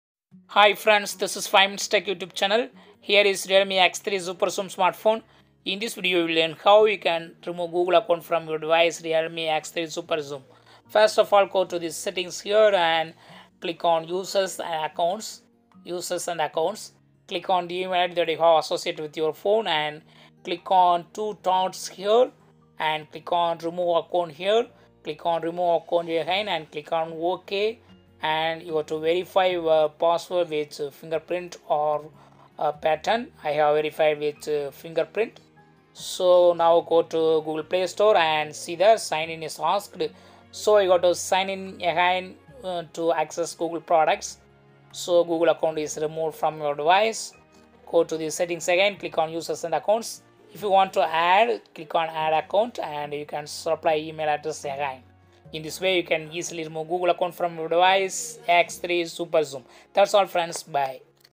How to remove Google account from Realme X3 / Realme X3 superzoom
Realme X3 / Realme X3 superzoom Google account remove settings: Learn here how to remove Google account from Realme X3 / Realme X3 superzoom smartphone